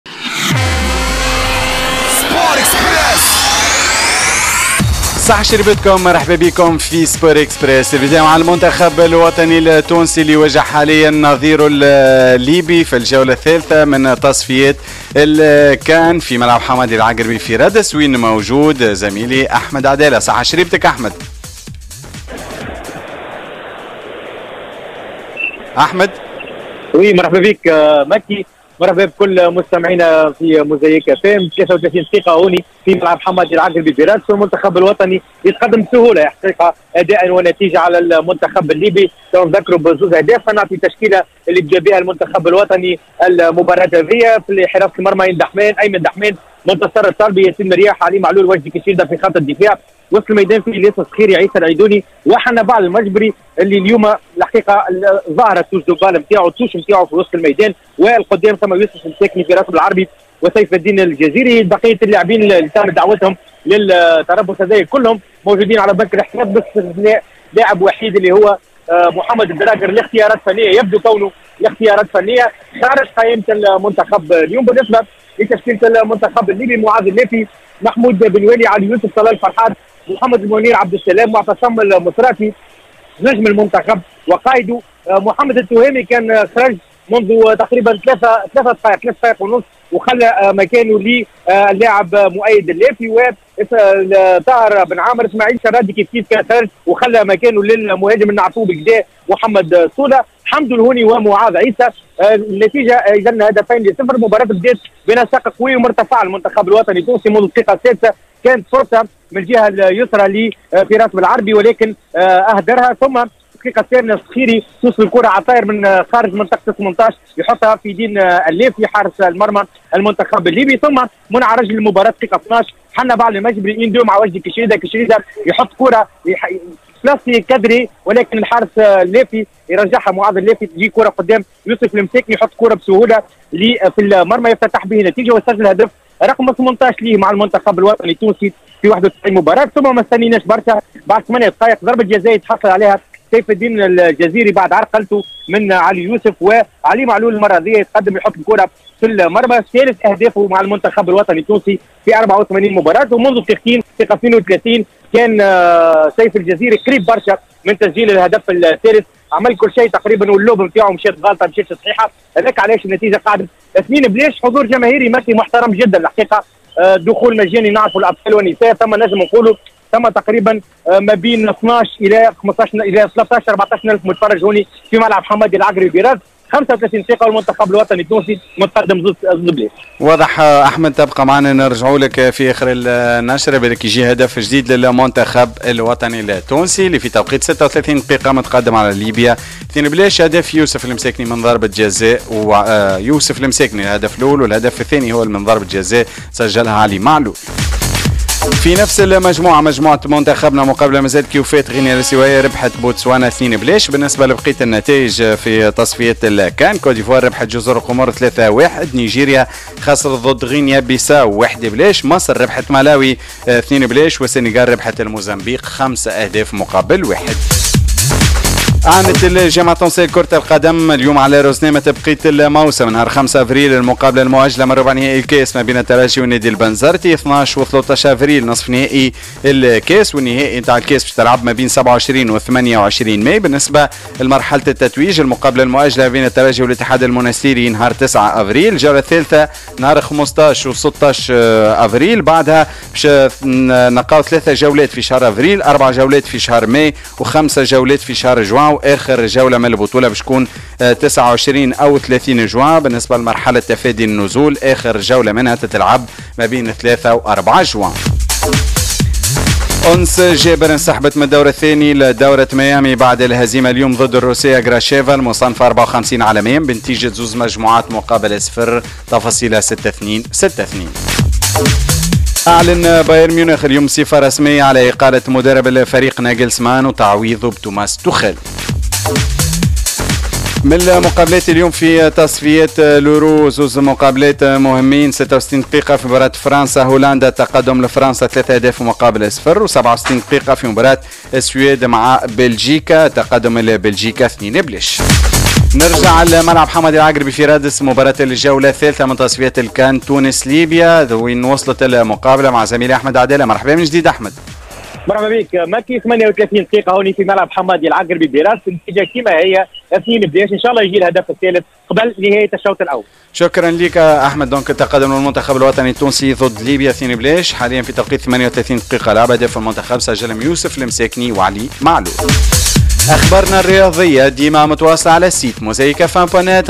Sport Express: متابعة مباشرة لمباراة المنتخب التونسي ضد ليبيا
- بعد طول انتظار الجامعة تعلن عن روزنامة بقية الموسم
- أنس جابر تودع دورة ميامي المفتوحة

إشترك الأن على قناة اليوتوب